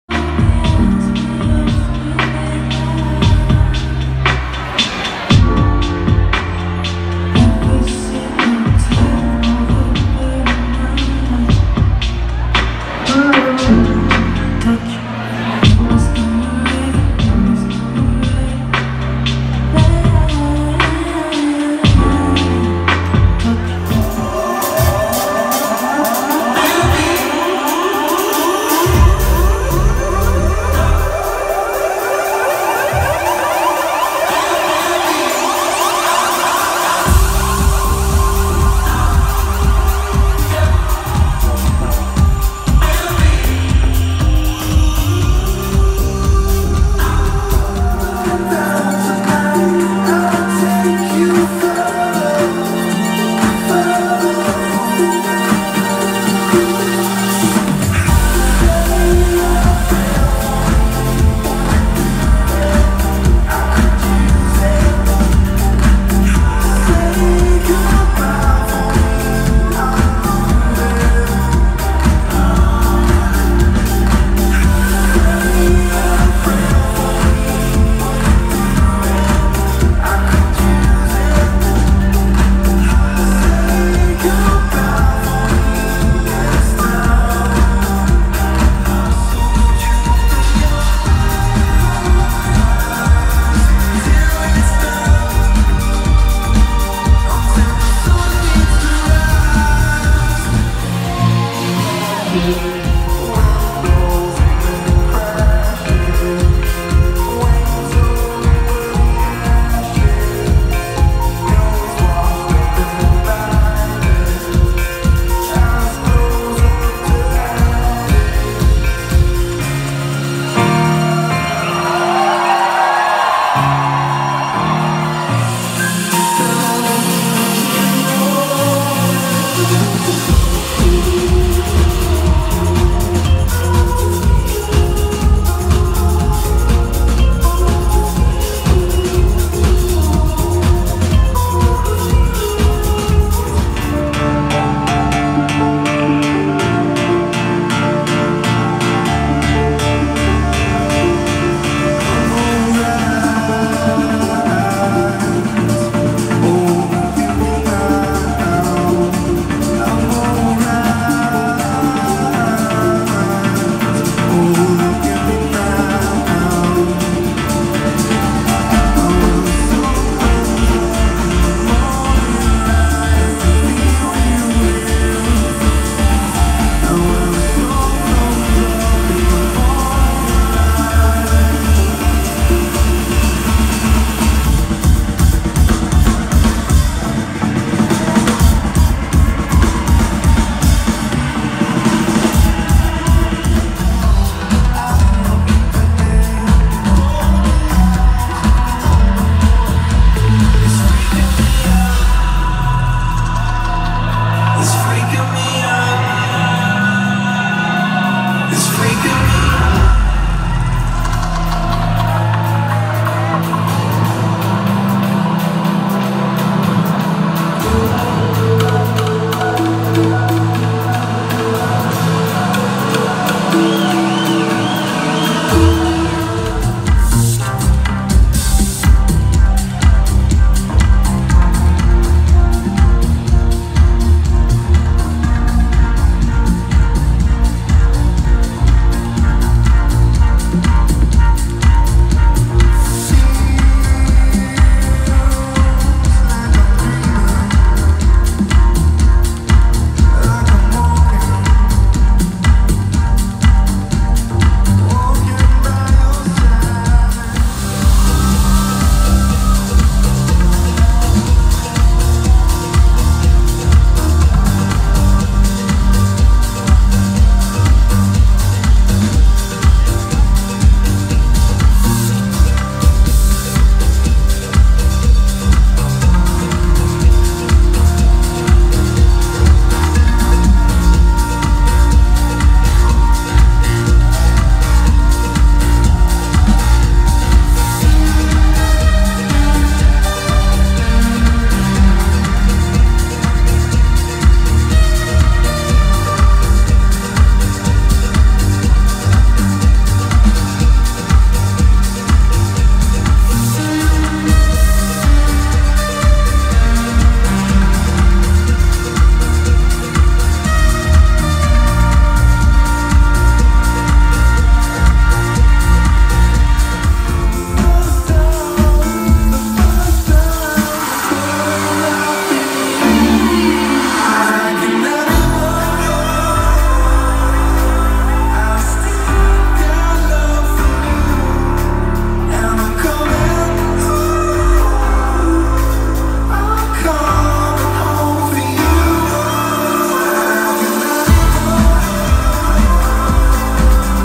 RÜFÜS at Wonder Ballroom Portland, OR
Just some short clips of the night.